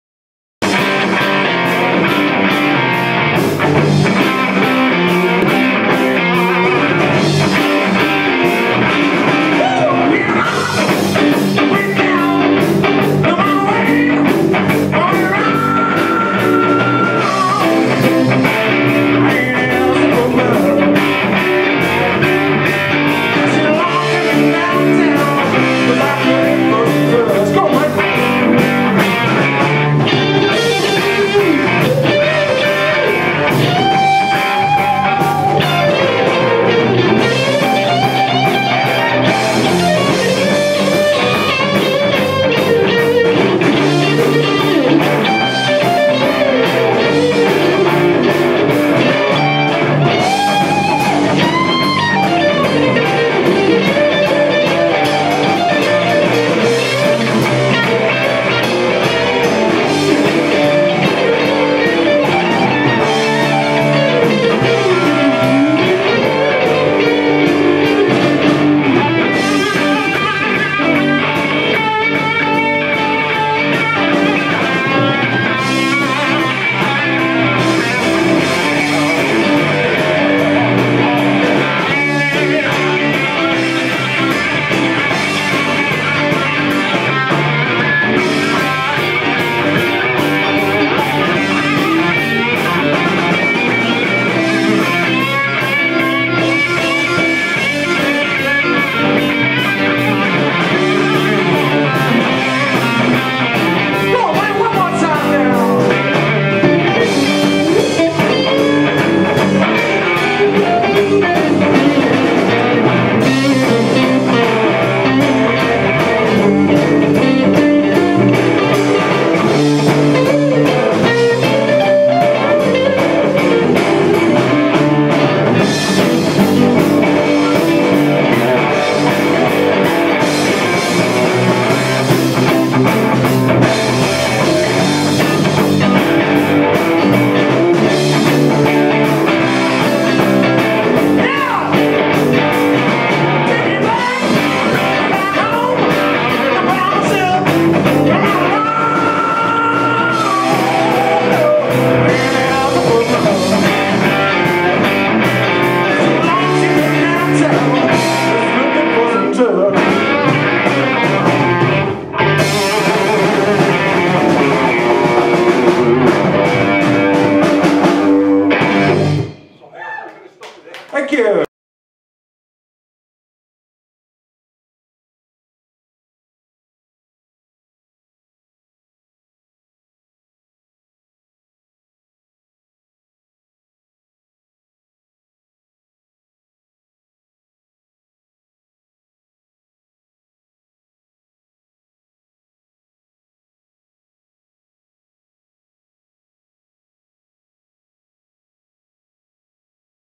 tush 30 10 2014
Blues Jam at the Woodman,  Every Thursday.  Keeping the Blues flame burning.